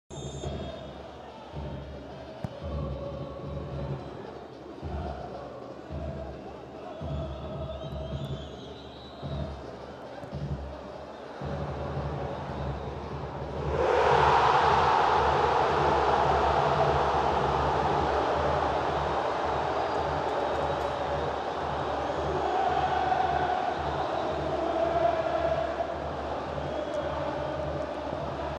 الوداد الرياضي 3-2 حسنية أكادير هدف حمزة أسرير في الدقيقة 80. البطولة_الإحترافية|الجولة13
الوداد الرياضي 3-2 حسنية أكادير هدف حمزة أسرير في الدقيقة 80.